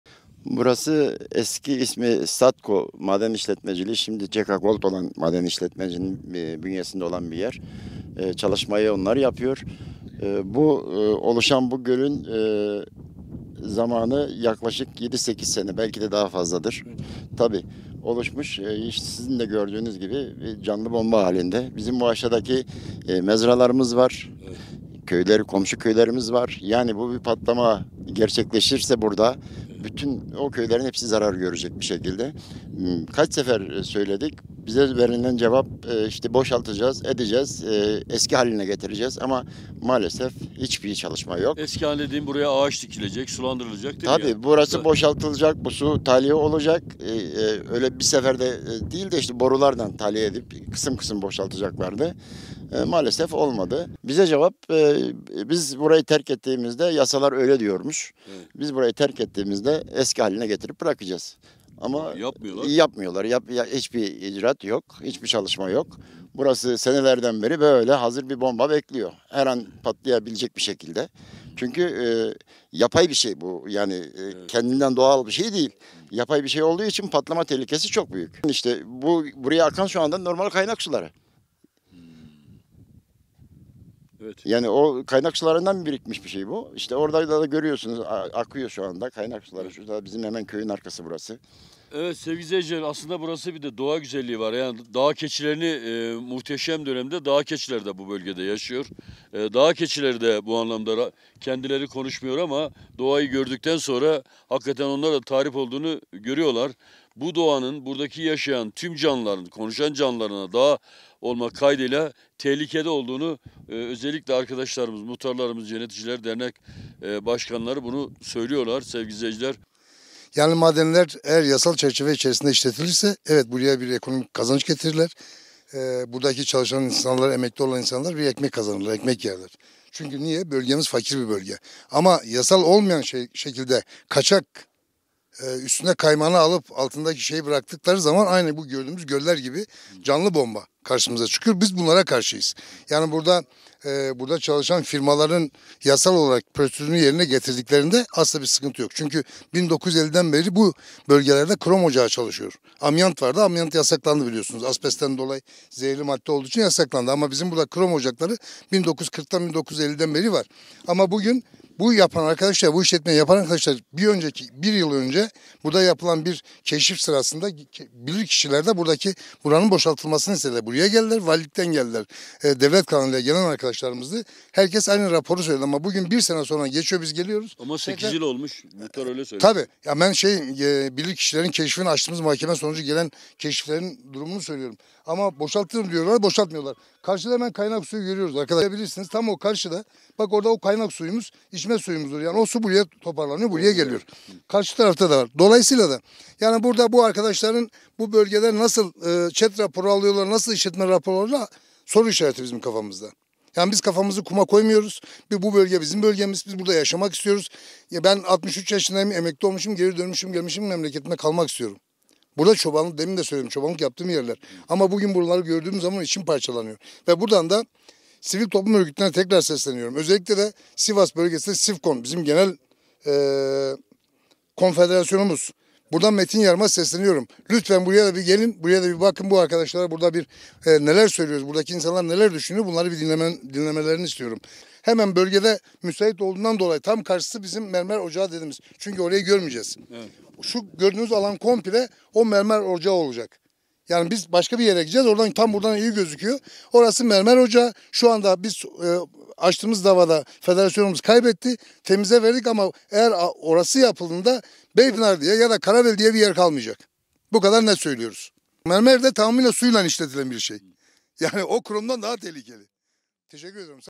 Sivas'ta maden şirketinin yapay göleti onlarca köyü tehdit ediyor
PİRHA-Sivas Karabel bölgesinde CK Gold Maden Şirketi tarafından çıkarılan krom madeninin yıkanması amacıyla yapılan yapay göletin her an patlayabileceği riski taşıdığı için köylüler endişe içinde. Maden şirketinin yaptığı yapay gölet herhangi bir deprem veya bir sel tehlikesiyle karşı karşıya kaldığı için dere kenarında bulunan bazı köyleri suyun altında bırakabileceği belirtildi. Büyük Köyü Muhtarı Mehmet Canpolat, göletle ilgili olarak “Su tahliye edilecekti ama olmadı. Senelerdir canlı bomba gibi bekliyor” dedi.